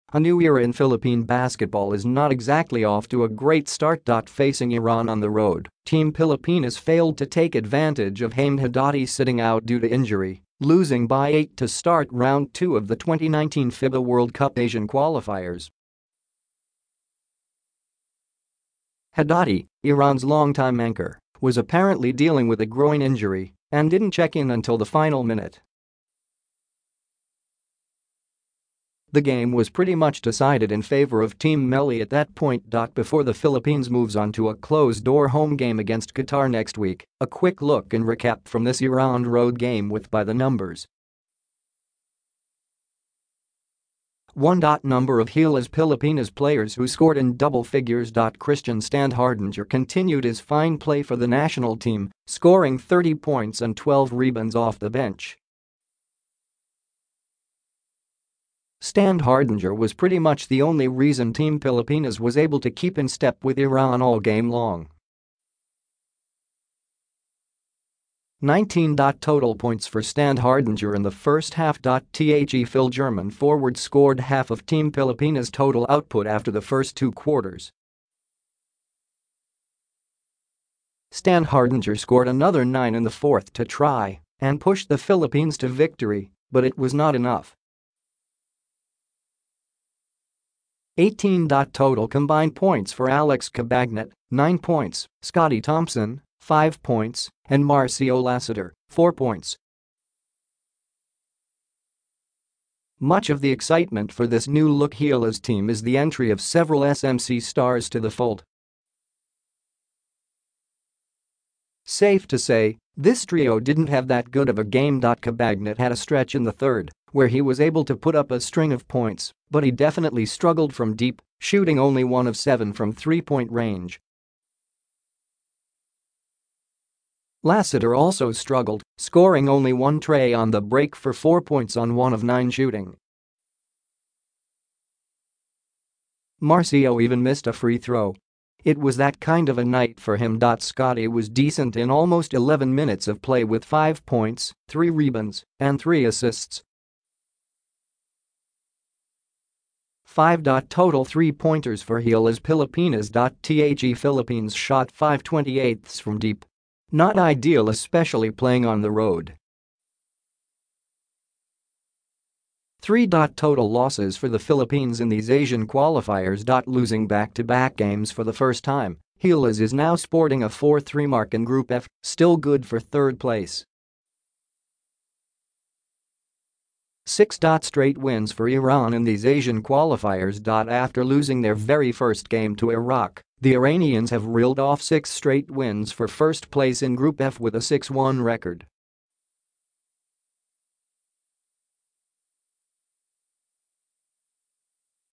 By the Numbers: New-look Team Pilipinas fires blanks to start new era
A new era in Philippine basketball is not exactly off to a great start.

Facing Iran on the road, Team Pilipinas failed to take advantage of Hamed Haddadi sitting out due to injury, losing by eight to start round 2 of the 2019 FIBA World Cup Asian Qualifiers.

Haddadi, Iran's longtime anchor, was apparently dealing with a groin injury and didn't check in until the final minute. The game was pretty much decided in favor of Team Melli at that point.

Before the Philippines moves on to a closed-door home game against Qatar next week, a quick look and recap from this Iran road game with By the Numbers.

1

Number of Gilas Pilipinas players who scored in double figures.

Christian Standhardinger continued his fine play for the national team, scoring 30 points and 12 rebounds off the bench.

Standhardinger was pretty much the only reason Team Pilipinas was able to keep in step with Iran all game long.

19

Total points for Standhardinger in the first half.

The Fil-German forward scored half of Team Pilipinas total output after the first two quarters. Standhardinger scored another nine in the fourth to try and push the Philippines to victory but it was not enough.

18

Total combined points for Alex Cabagnot (9 points), Scottie Thompson (5 points), and Marcio Lassiter (4 points).

Much of the excitement for this new-look Gilas team is the entry of several SMC stars to the fold. Safe to say, this trio didn't have that good of a game.

Cabagnot had a stretch in the third where he was able to put up a string of points but he definitely struggled from deep, shooting only 1-of-7 from three-point range.

Lassiter also struggled, scoring only one trey on the break for four points on 1-of-9 shooting. Marcio even missed a free throw. it was that kind of a night for him.

Scottie was decent in almost 11 minutes of play with five points, three rebounds, and three assists.

5

Total three-pointers for Gilas Pilipinas.

The Philippines shot 5/28 from deep. Not ideal especially playing on the road.

3

Total losses for the Philippines in these Asian Qualifiers.

Losing back-to-back games for the first time, Gilas is now sporting a 4-3 mark in Group F, still good for third place.

6

Straight wins for Iran in these Asian Qualifiers.

After losing their very first game to Iraq, the Iranians have reeled off six straight wins for first place in Group F with a 6-1 record.